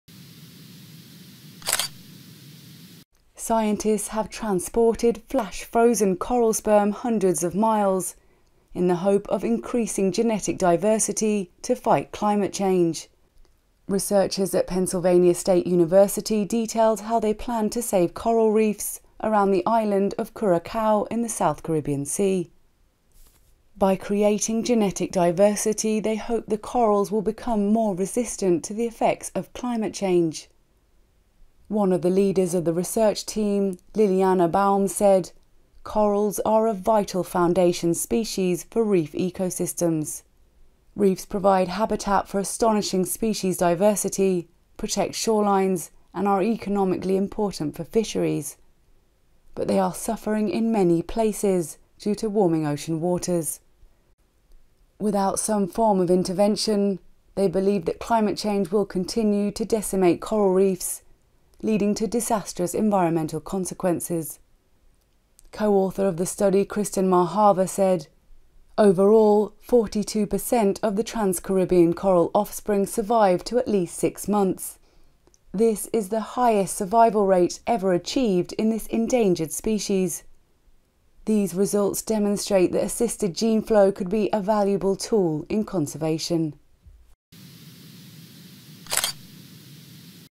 Sperm Donor Coral To Help Tackle Climate Change
Scientists have transported flash-frozen coral sperm over hundreds of miles to fertilize eggs in the hope that by increasing genetic diversity the corals will become more resistant to the effects of climate change which are decimating coral reefs across the planet.

In a statement released by Pennsylvania State University Eberly College of Science researchers detailed how they plan to save coral reefs around the island of Curacao in the southern Caribbean Sea by fertilizing eggs with Coral sperm flash-frozen and transported from Florida and Puerto Rico.

Iliana Baums, professor of biology at Penn State and one of the leaders of the research team, said: “Corals are a vital foundation species for reef ecosystems.”

Baums added: “Reefs provide habitat for astonishing species diversity, protect shorelines, and are economically important for fisheries, but they are suffering in many places due to warming ocean waters.”

According to the Fish & Wildlife Foundation of Florida, Coral reefs occupy less than 1 per cent of the world’s oceans, but approximately 25 per cent of marine fish species inhabit coral reefs during some part of their lives.

The researchers believe that without some form of intervention climate change will continue to decimate coral reefs leading to disastrous environmental consequences.

According to the statement, genetic diversity fuels the ability of a species to adapt to challenging circumstances.

The researchers stated: “One of the main sources of genetic diversity is sexual reproduction—new combinations of genes are created when a sperm fertilizes an egg.”

They found that sexual reproduction by Elkhorn corals in the Caribbean is now extremely rare and due to the stationary nature of coral they have little chance of gaining diversity through interaction between distant reefs making them extremely vulnerable to the effects of global warming.

Corals reproduce by sending bundles of eggs and sperm into the water in a huge underwater event that takes place during full moons.

Kristen Marhaver, a marine biologist at the CARMABI Foundation in Curaçao and co-corresponding author of the study. said: “Overall, 42% of the trans-Caribbean coral offspring survived to at least six months, the highest survival rate ever achieved in this endangered species,”

Baums said: “These results demonstrate that assisted gene flow could be a valuable tool in conservation by introducing genetic diversity into this critically endangered marine species.”

The university released the statement titled ‘Long-distance relationships for endangered corals’ on Tuesday (7th September).

The researchers collected these bundles in Florida and Puerto Rico, separated the eggs and the sperm and then ‘flash-froze’ the sperm using a cutting edge liquid nitrogen cryopreservation technique.

Baums said: “Because these corals only produce eggs and sperm once per year, frozen sperm collected in Florida and Puerto Rico needed to be cryopreserved in advance and stored for over a year until it could be used for a spawning event in Curacao,”

Baums said: “To increase genetic diversity in corals, we can use ‘assisted gene flow’ by bringing corals together that are physically distant in the wild, but this is logistically incredibly difficult.”

Mary Hagedorn, a research biologist at the Smithsonian Conservation Biology Institute and lead author of the study, said: “Most corals only attempt sexual reproduction once a year and the eggs and sperm are only viable for a short time period.”

Hagedorn, who developed the technique to cryopreserve coral sperm, added: “Cryopreservation allows us to breed corals with parentage from hundreds of miles apart.”